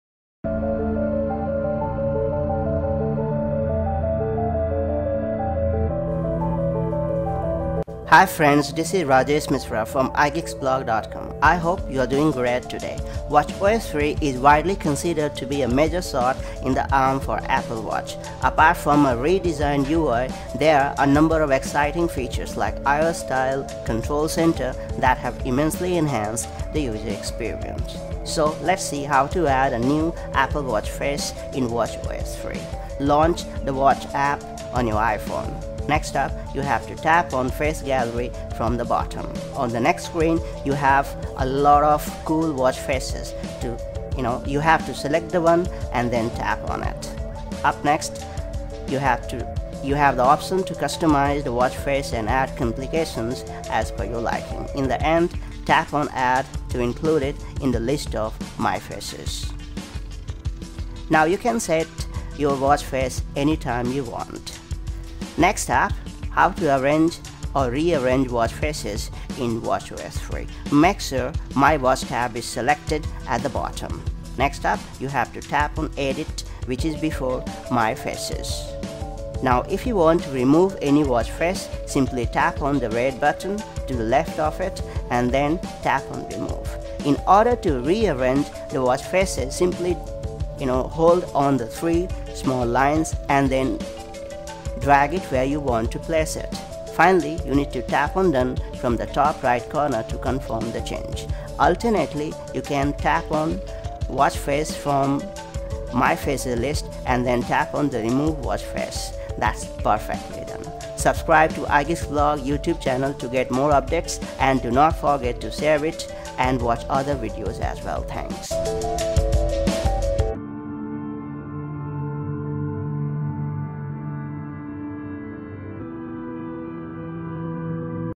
How to Add/Remove/Rearrange Apple Watch Faces on watchOS 3
With watchOS 3 installed on your Apple Watch, you can now add, remove and even rearrange Watch Faces right from your paired iPhone. Check out this quick video for a perfect guide.